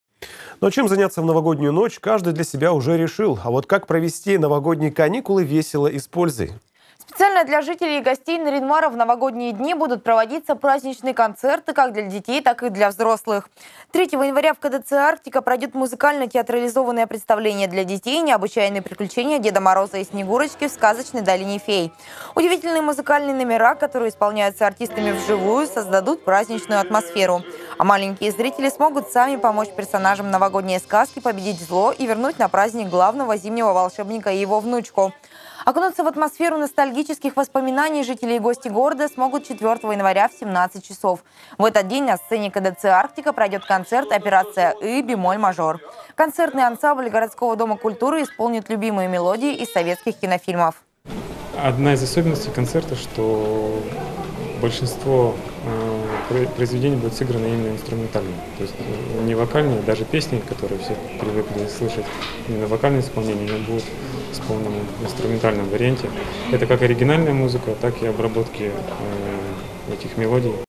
Нарьян-марский Дом культуры готовится к насыщенной новогодней программе
Как провести новогодние каникулы весело и интересно? Для жителей и гостей Нарьян-Мара в новогодние дни будут проводиться праздничные концерты. Так 3 января в КДЦ «Арктика» пройдет музыкально-театрализованное представление «Необычайные приключения Деда Мороза и Снегурочки в сказочной долине фей».